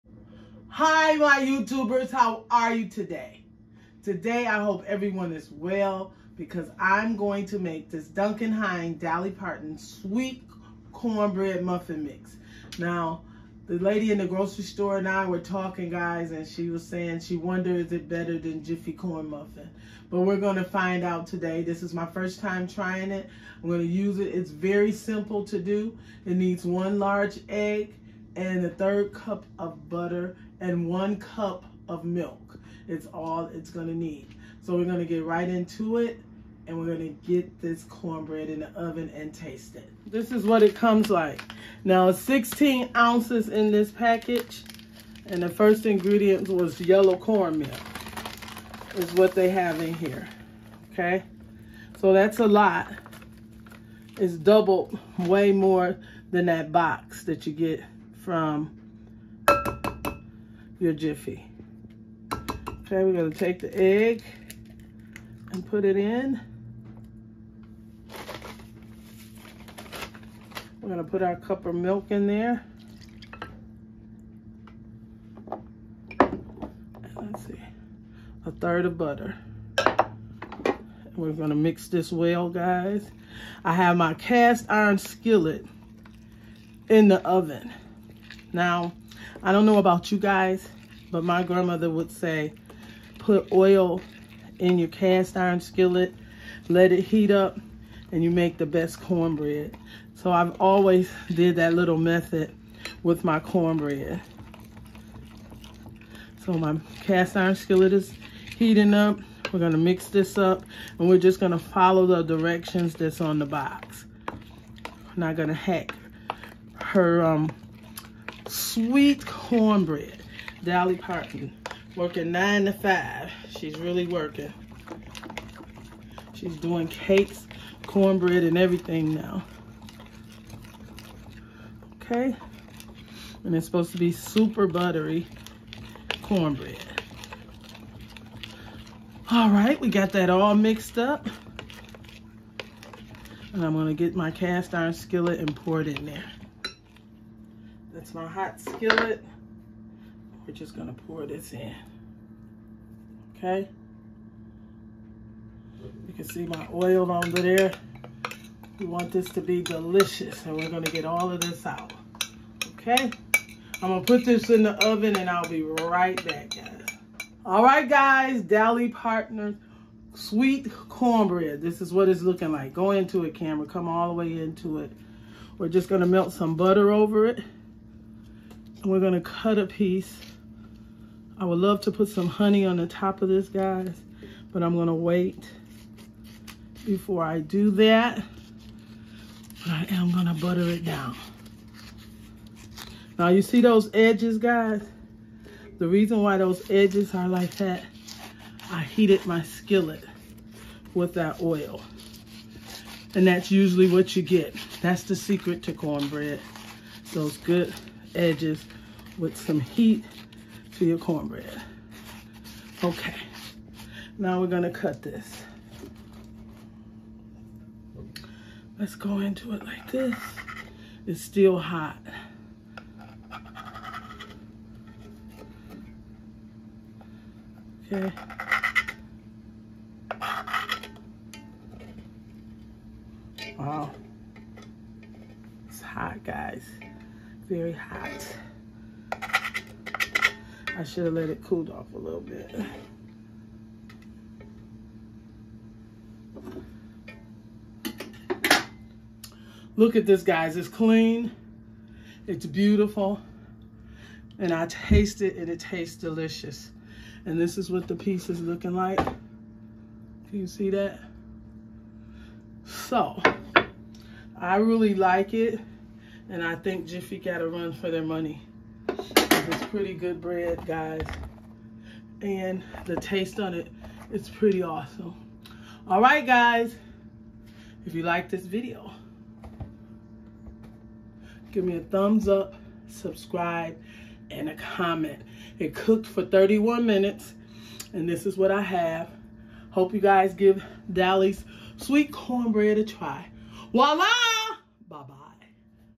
Dolly Parton's Sweet Cornbread And Muffin Mix (Duncan Hines)
Today I made Dolly Parton's Cornbread just because I could, and it was straightforward to make and delicious. You must try it!

Ingredients

1 16oz Dolly Parton's Sweet Cornbread Muffin Mix
1 Egg
1 Cup of Milk
 ⅓ Butter
1 Hot Skillet 375° to 400°